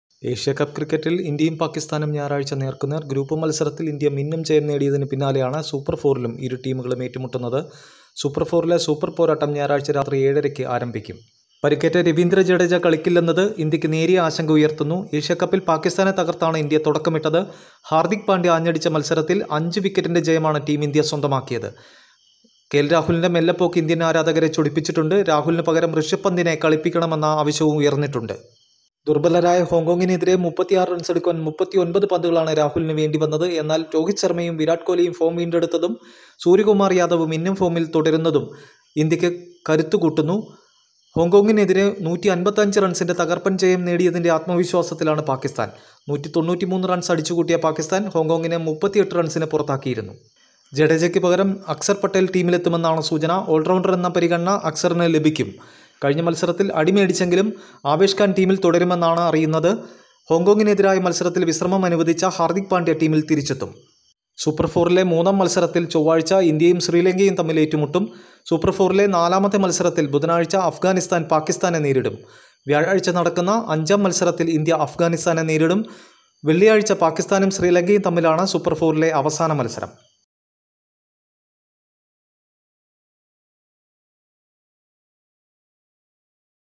രവീന്ദ്ര ജഡേജയ്ക്ക് പകരം അക്സര്‍ പട്ടേല്‍, ഹാര്‍ദ്ദിക് പാണ്ഡ്യ തിരിച്ചെത്തും I india - pakistan
ഏഷ്യാകപ്പില്‍ വീണ്ടും ഇന്ത്യയും- പാകിസ്ഥാനും നേര്‍ക്കുനേര്‍